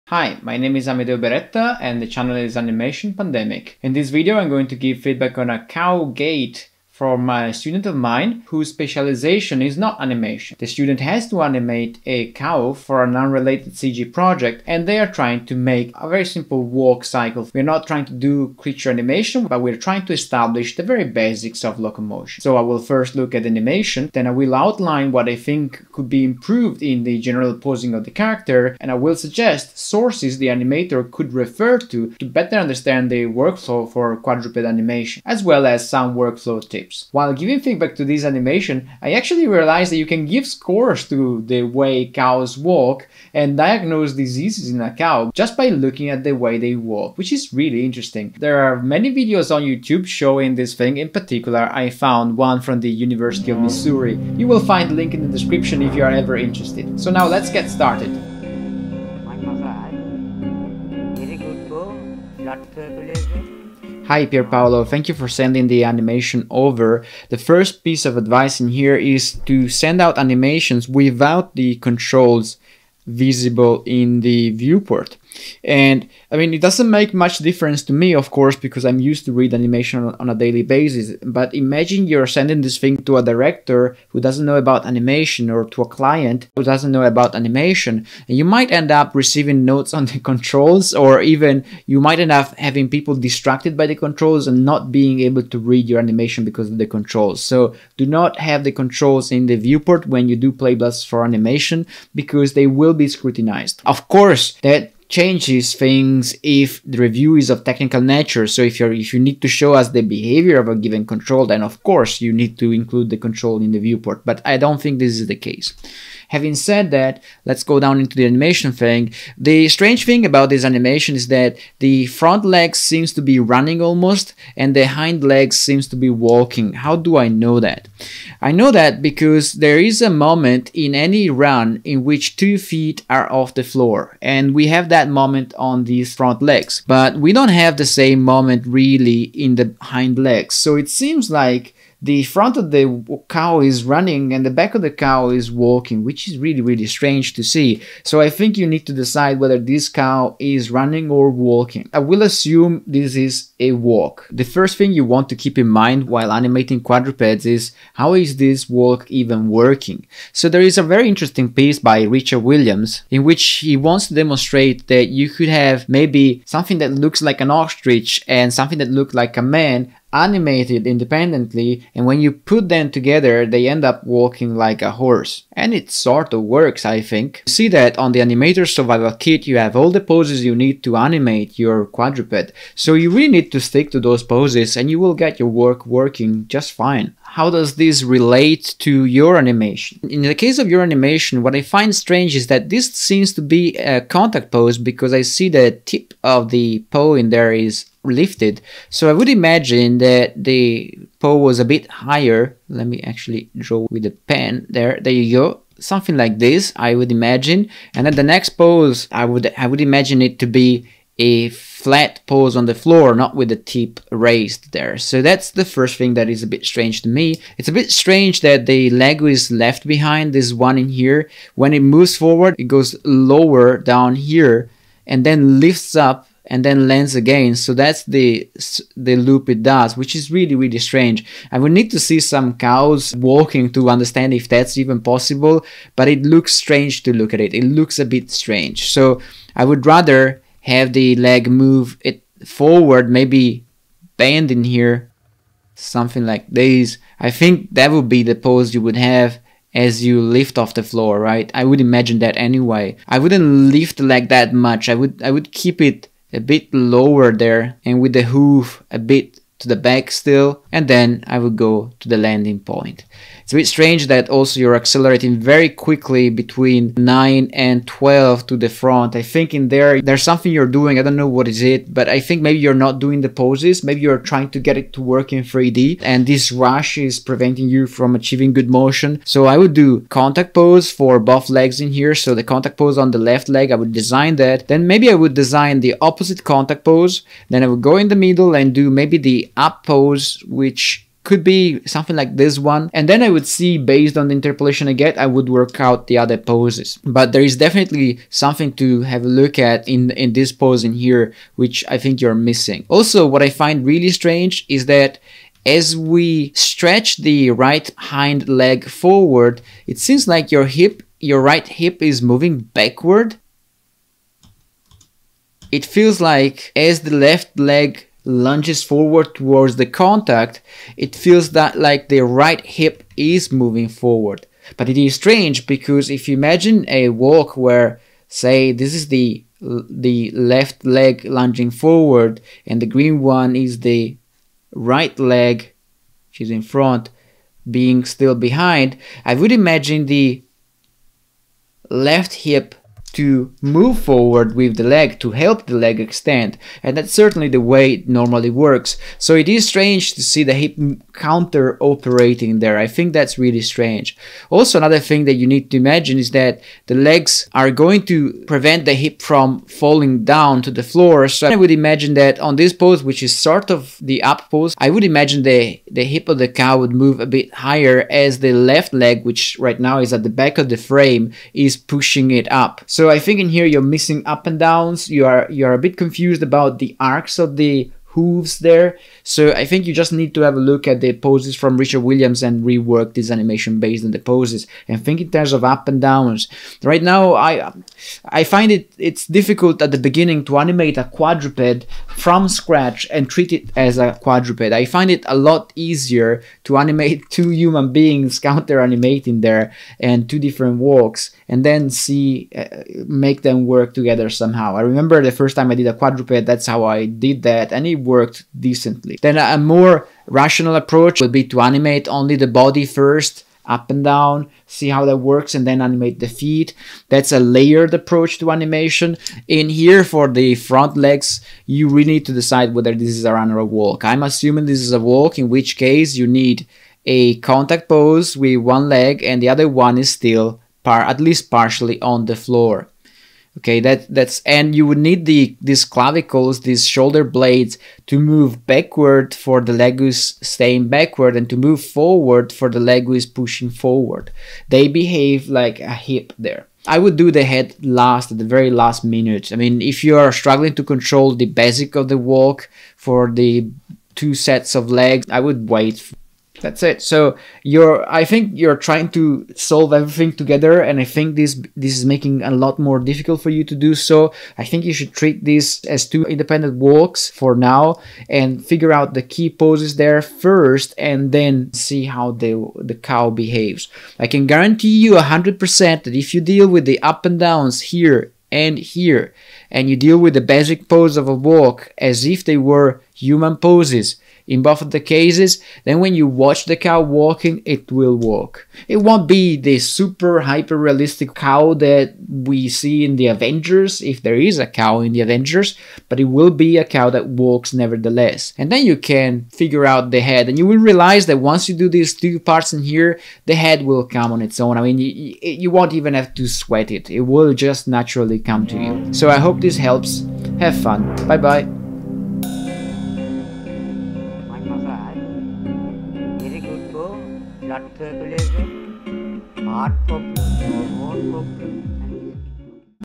What's odd about this cow walk? Animating a quadruped walk
Feedback video on a cow walk that does not need to be super realistic, but still needs to work.